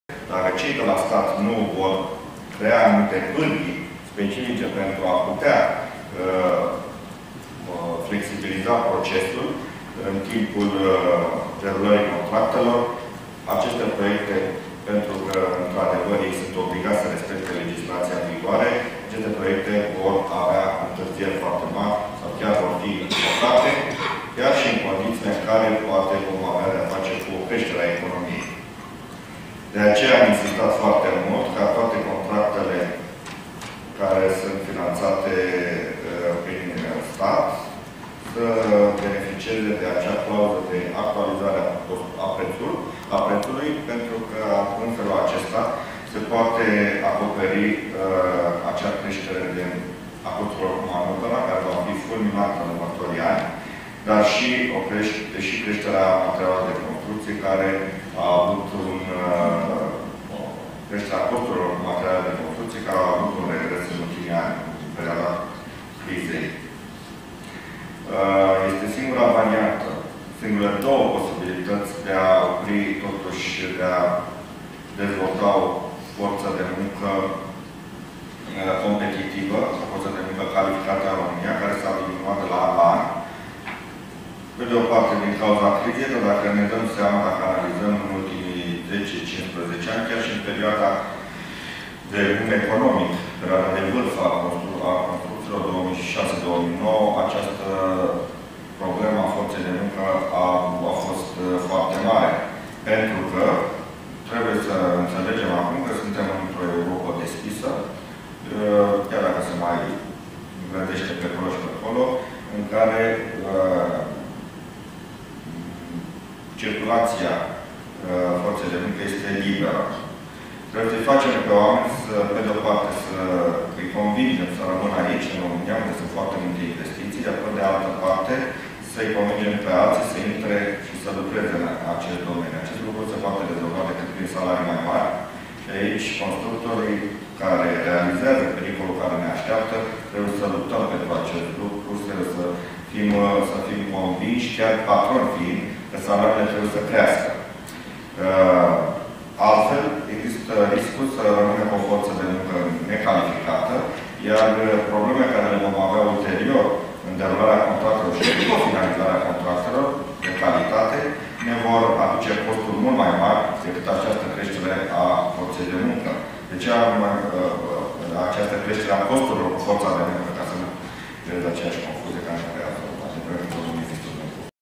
Erbaşu: Proiectele de infrastructură riscă să fie blocate
Conferinţă CAPITAL. Erbaşu: Proiectele de infrastructură riscă să fie blocate dacă nu se preconizează o creştere a salariilor în construcţii